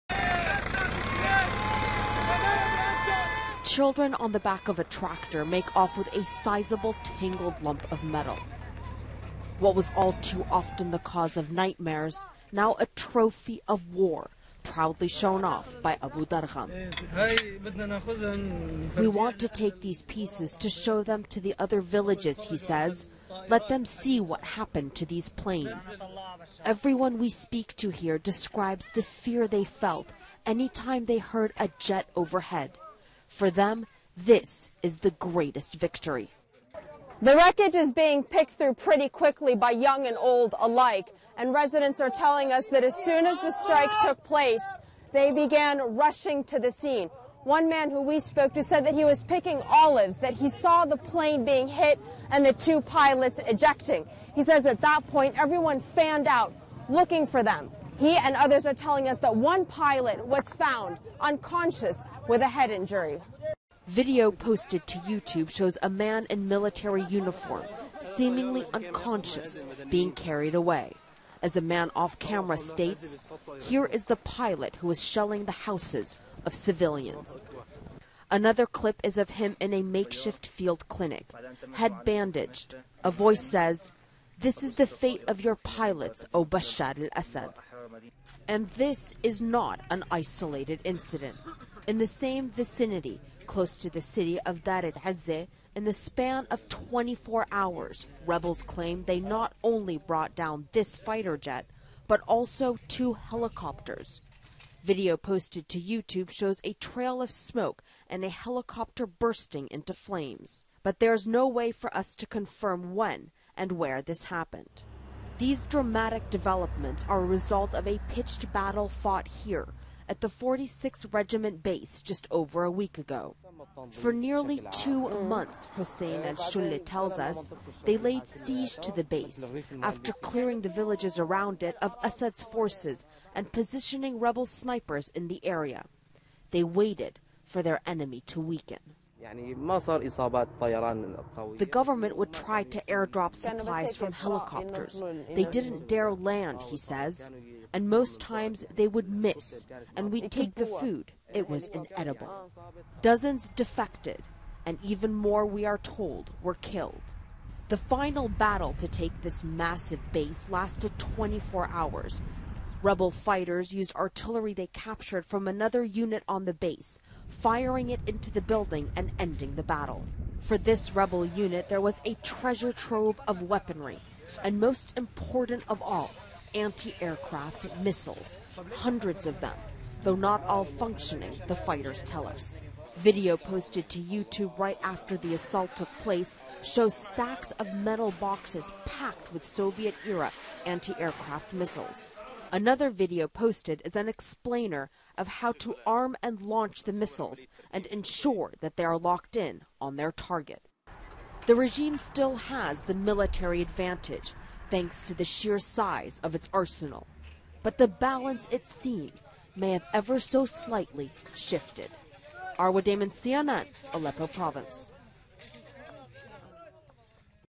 CNN's Arwa Damon Documents Rebel Progress In Syria
Villagers cheer as word spreads of a downed military jet in Syria, one of three rebels claim to have shot down in 24 hours. Video uploaded to YouTube shows an injured man being carried away. CNN's Arwa Damon reports.

Rebel leader says Syria on path to becoming Islamic state (AP, Nov 30, 2012):
"A leader of an al-Qaida-inspired militant group fighting the regime in Syria said his men do not fear death and they are determined to form an Islamic state. Jabhat al-Nusra — Arabic for "the Support Front" — has claimed responsibility for suicide bombings and other attacks on regime targets across the country.The group has raised fears of a growing Islamic militant element among the forces seeking to topple President Bashar Assad. "Thanks to our strong faith we do not fear death, because we think that if you are killed by the hands of this regime, then we will be martyrs and we will go to paradise," said Sheik Abu Ahmed, 41, a regional military commander for al-Nusra in the northern Hasaka region. "We want Sharia (Islamic law) to be applied because it's the right path for all humanity," he added. "All these constitutional laws couldn't realize the people's happiness.""

"The main road to the airport of the Syrian capital Damascus has been closed and there have been no flights since early morning, as rebel groups reported fierce fighting in the area. Emirates Airlines and Egypt Air cancelled flights to Damascus because of the "deteriorating situation"."

Syrian villagers cheer downed jet; rebels display captured missiles

Rebel Arms Flow Is Said to Benefit Jihadists in Syria

Citing U.S. Fears, Arab Allies Limit Syrian Rebel Aid